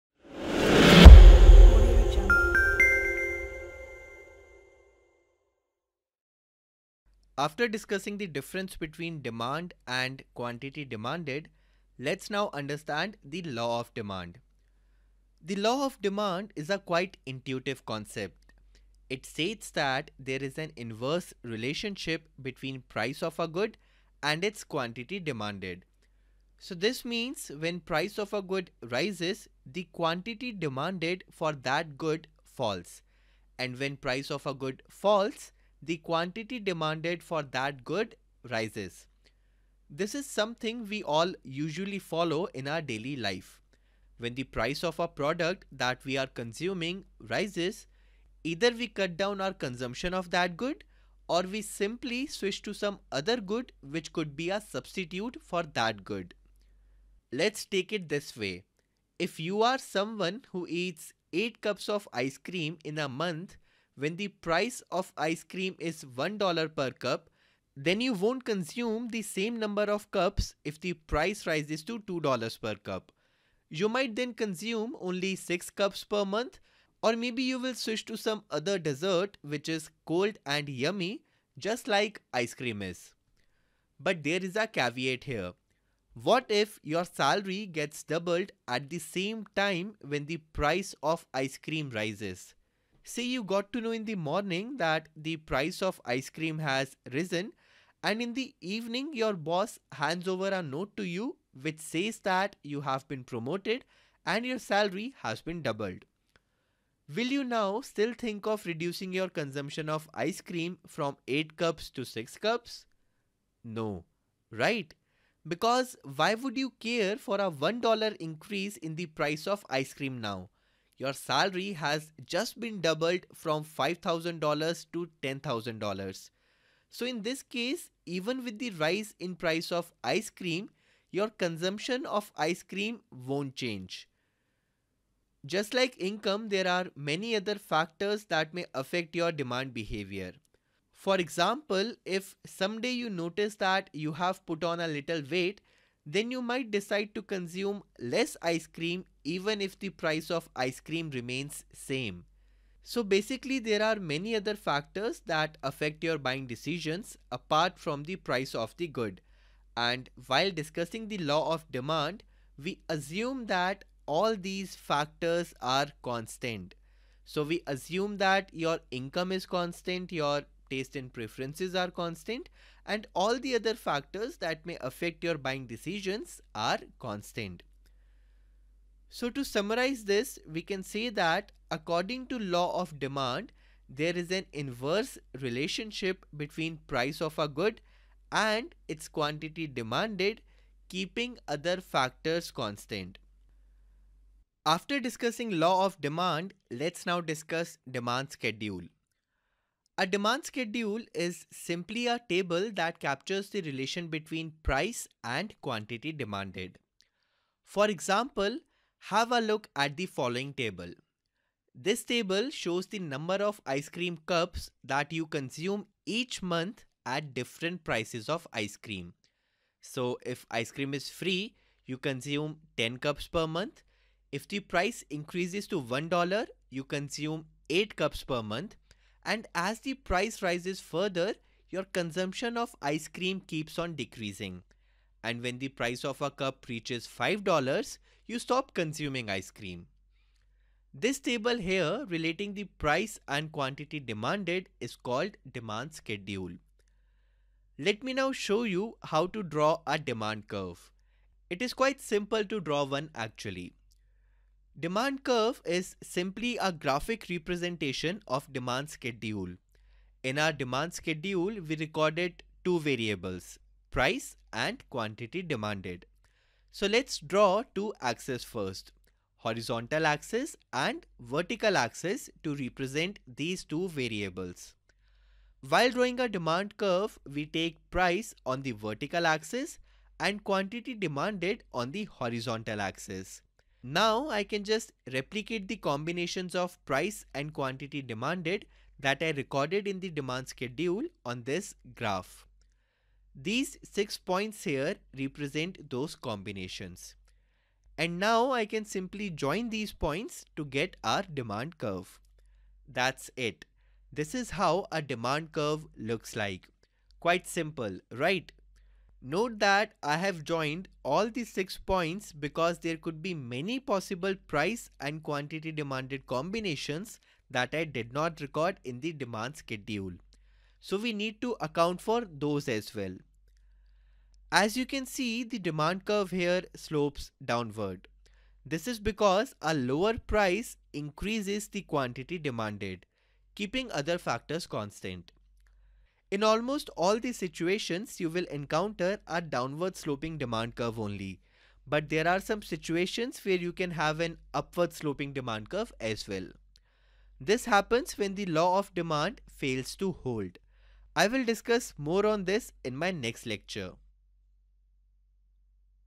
Law of Demand and Demand Curve
This lecture is a part of a 1 hour course on Demand & Supply Mechanism and Market Equilibrium.

In this course, I will take you through the heart of Economics - Demand and Supply Mechanism (Determined by Buyers and Sellers ) and we will discuss the following topics :

What is Demand?
Do you demand less when the price of a commodity rises.? Yes..? Think again..!
Law of Demand, Demand Schedule and Demand Curve
Movements and Shifts in Demand Curve
Is your Demand only a function of price?
What is supply?
Law of Supply , Supply Schedule and Supply Curve
Movements and Shifts in Supply Curve
What affects supply of a good in the market?
We, as buyers, want to pay as low a price as possible; sellers, on the other hand, want to charge as high a price as possible, seems like there are opposite interests here, then who gets to decide the price?
How does this price mechanism function?

It is a beginner level course and all the topics and concepts start from scratch, so even if you are not familiar with Economics , you may take this as your first step to understand this powerful subject. Moreover, it is FREE, so what are you waiting for..!